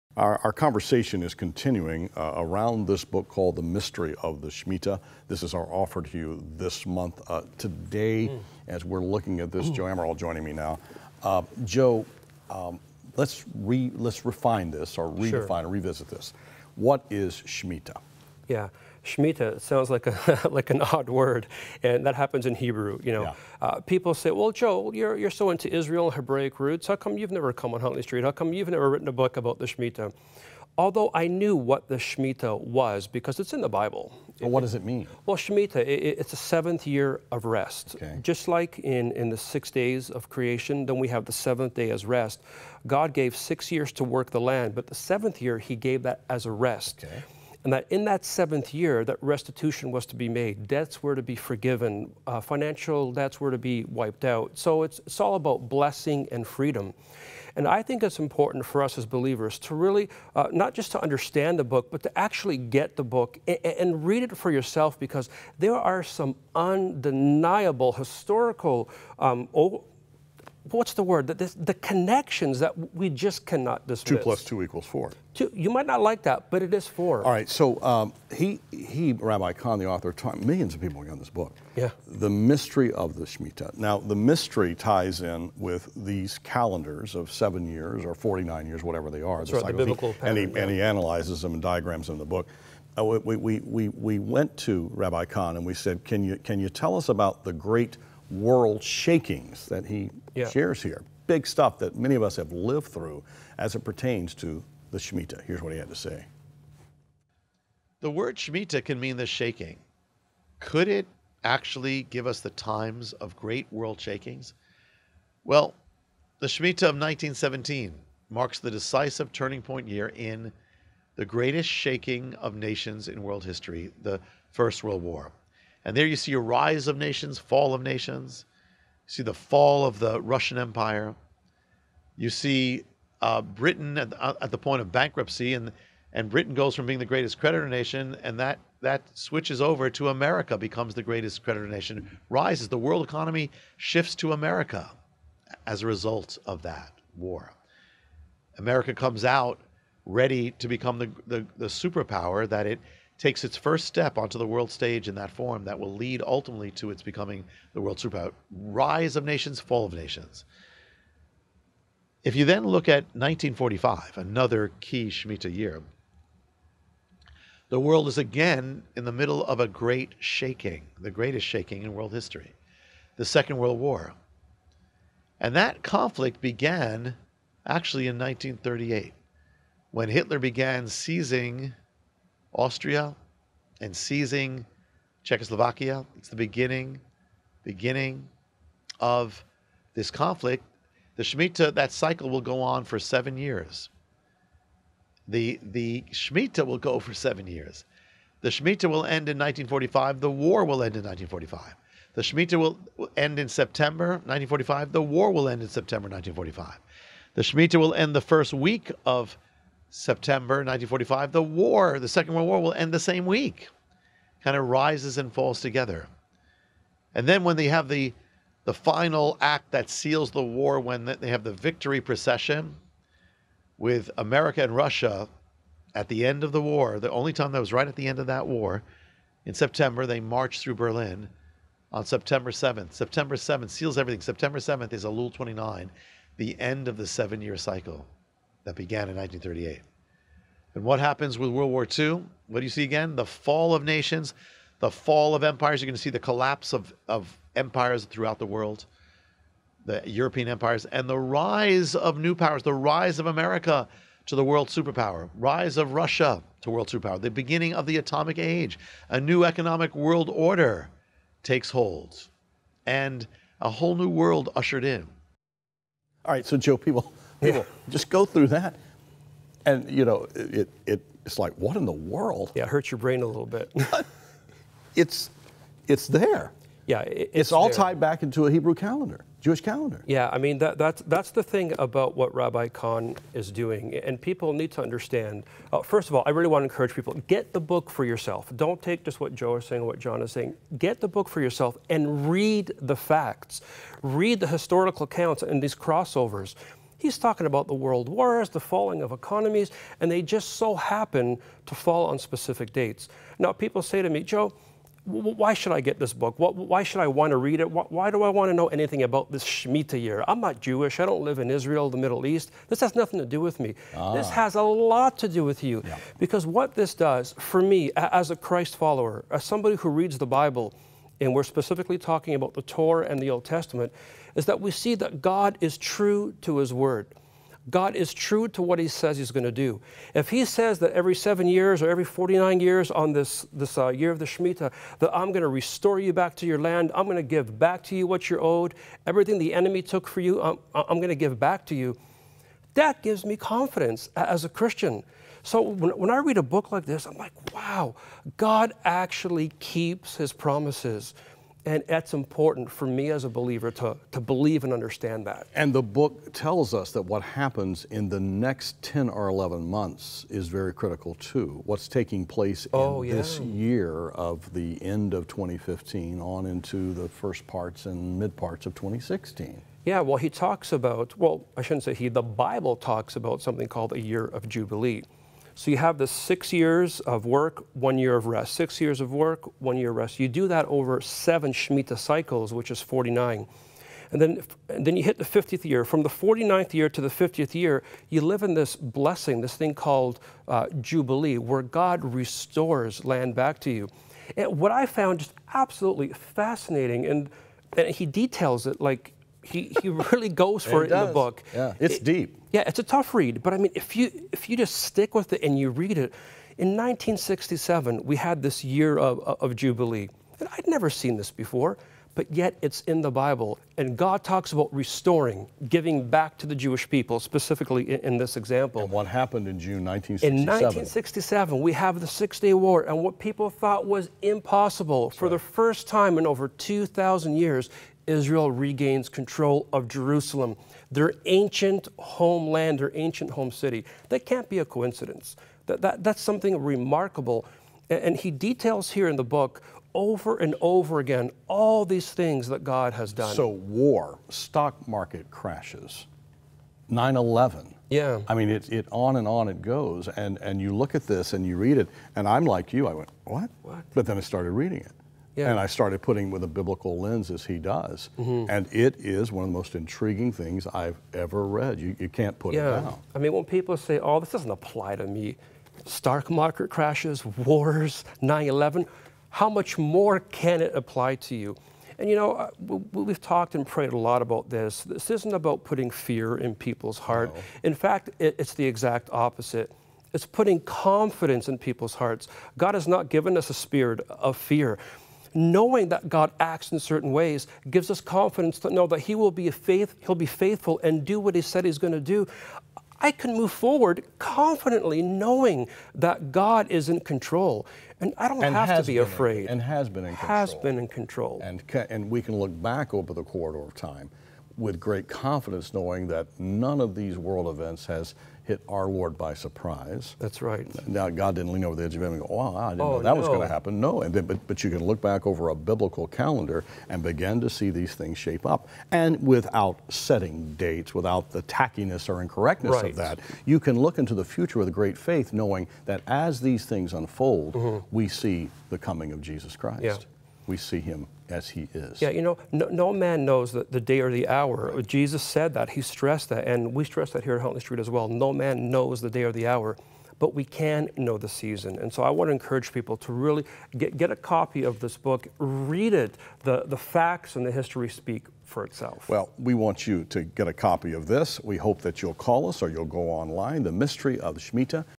Great World Shakings as it Pertains to the Shemitah
Joe Amaral and John Hull delve deeper into the Mystery of the Shemitah
The Mystery of the Shemitah:
Expanding on a mystery introduced in his bestseller The Harbinger, messianic Rabbi Cahn explains the prophetic significance of the "Shemitah" a recurring Sabbath year prescribed by the Torah. Learn how the ancient 7-year milestone is connected to world history, including the events of the Great Depression, 9/11, and possibly the fall of the United States.